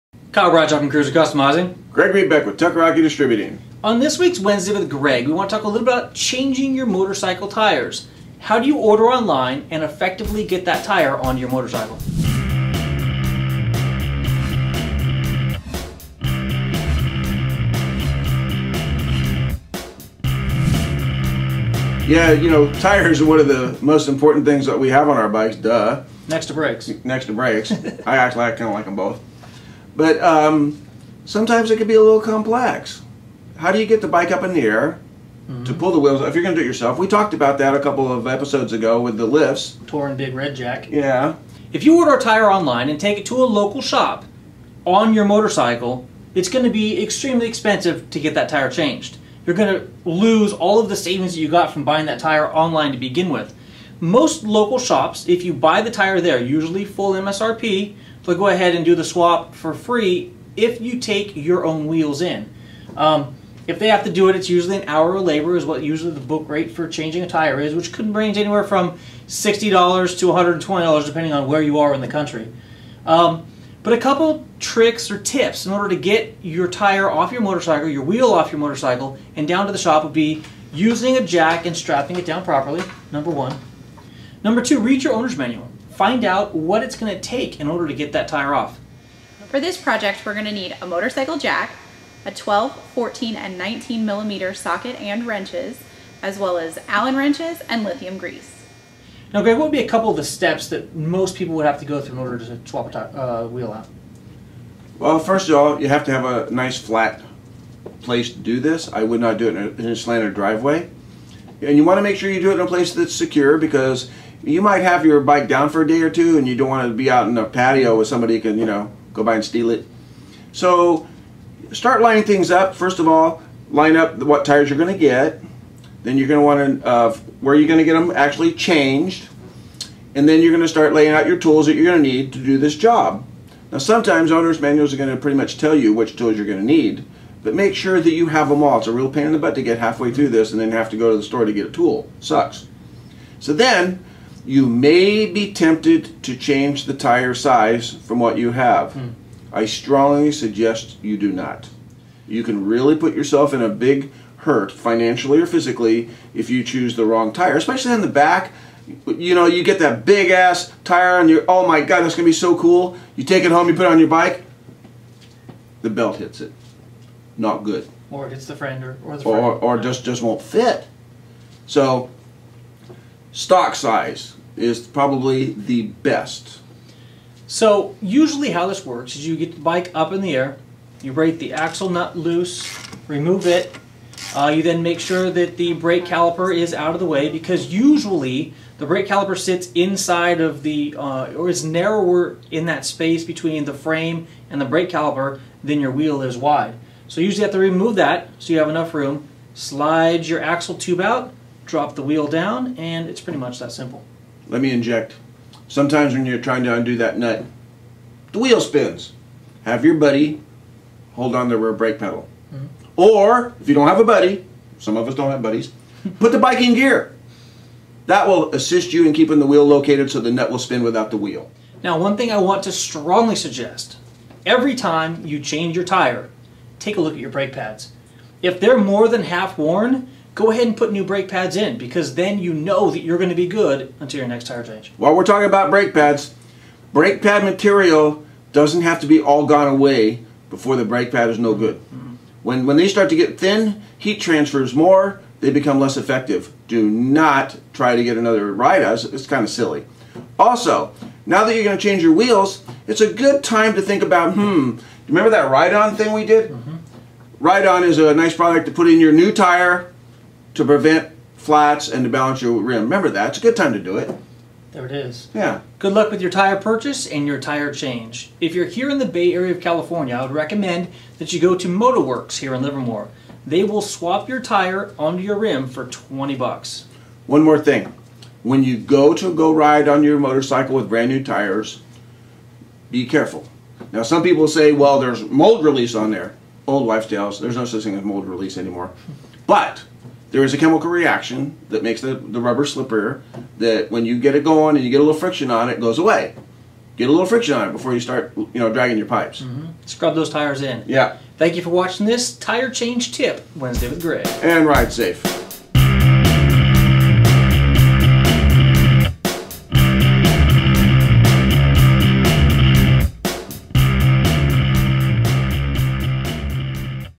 WWGe113 Motorcycle Tire Change Tips
In this Wednesday With Greg episode 113, Kyle Bradshaw of discusses tips and things to think about when changing your motorcycle tires with Greg Beck of Tucker Rocky.

Kyle & Greg